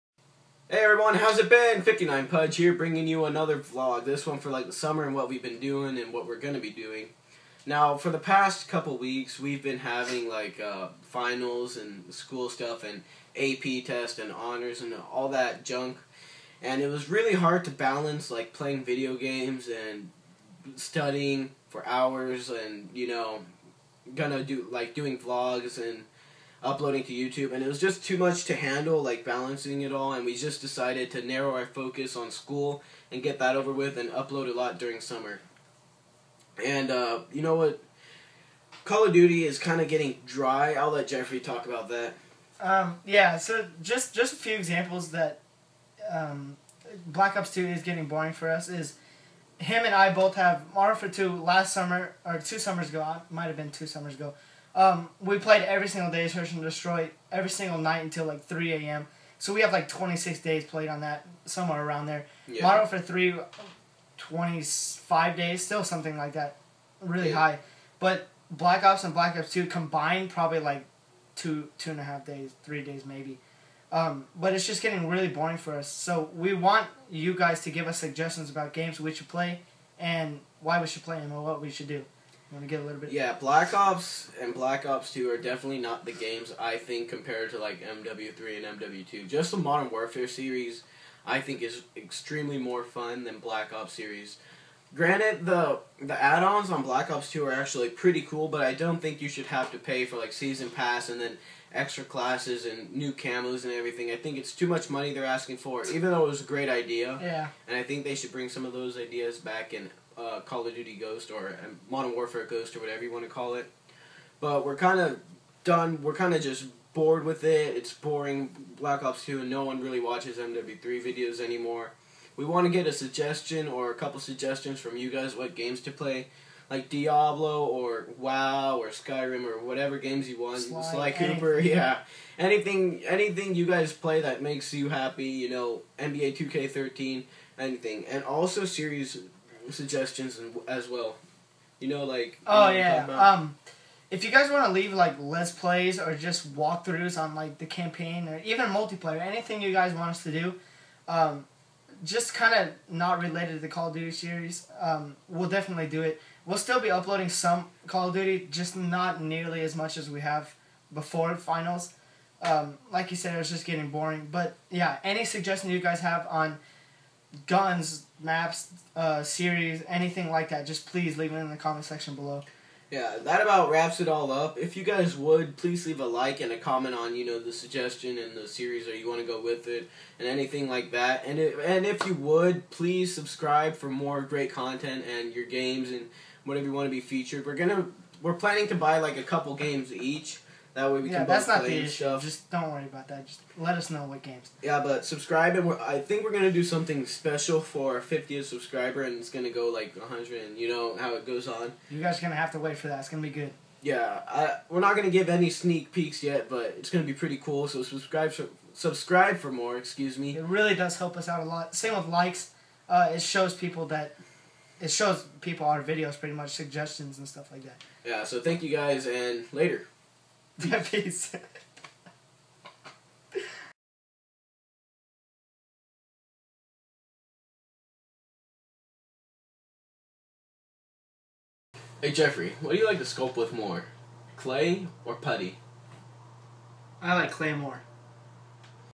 Summer Vacation Vlog
Whaddup guys, 59pudge here bringing you a vlog about our summer and why we haven't uploaded over the past month. Thanks so much for all the support, and look forward to great upcoming content.